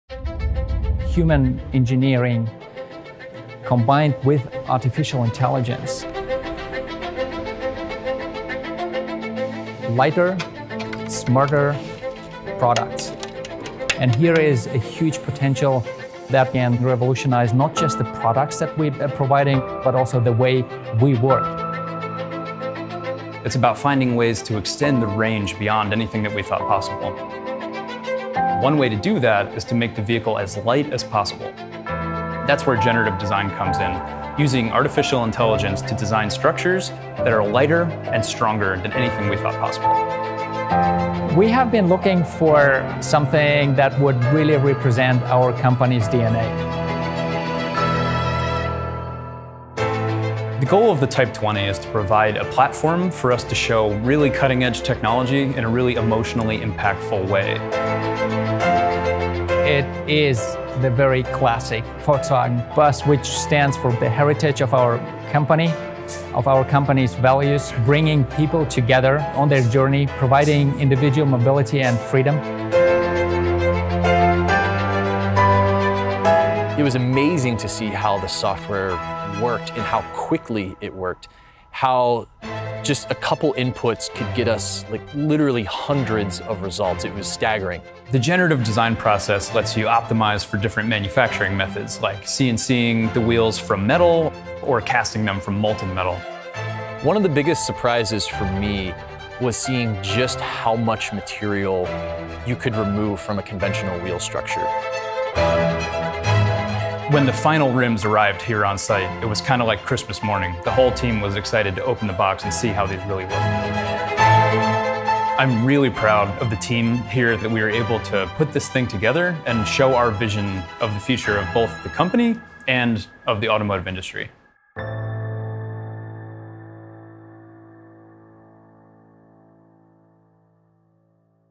Volkswagen Concept Bus
Autodesk collaborates with Volkswagen Group using generative design to lightweight parts on a unique vehicle concept that combines VW heritage with futuristic technology.